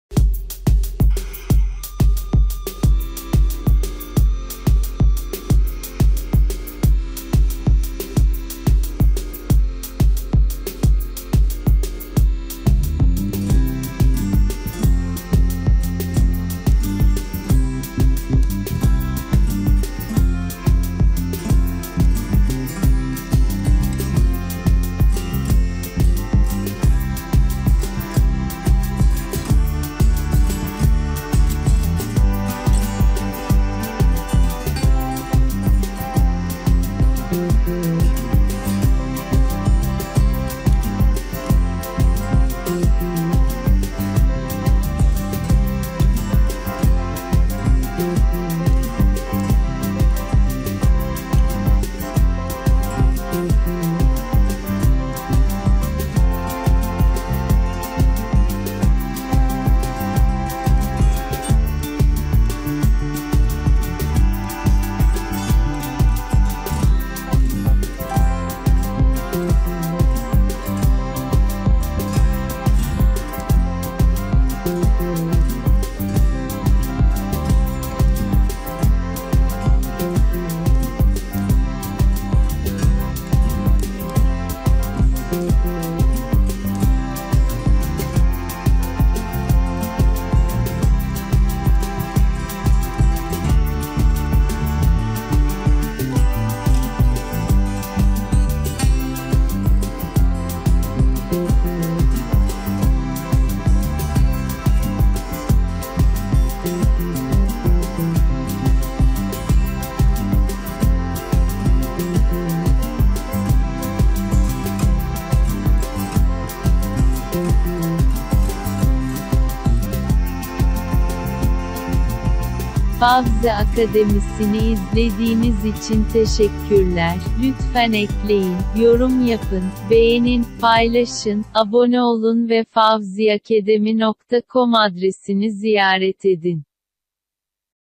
(TR) Apple Watch unuzda Walkie-Talkie yi kullanma ?
Sana göstereceğim. Apple Watch'ınızda El Telsizi modu nasıl kullanılır.Telsizli elma izle dizime kişileri nasıl eklerim?

FawziAcademy Web Sitesini ziyaret etmek için buraya tıklayın.

Fawziacademy google tercümanı kullan ve google translate uygulamasını çevirmek ve seslendirmek için kullan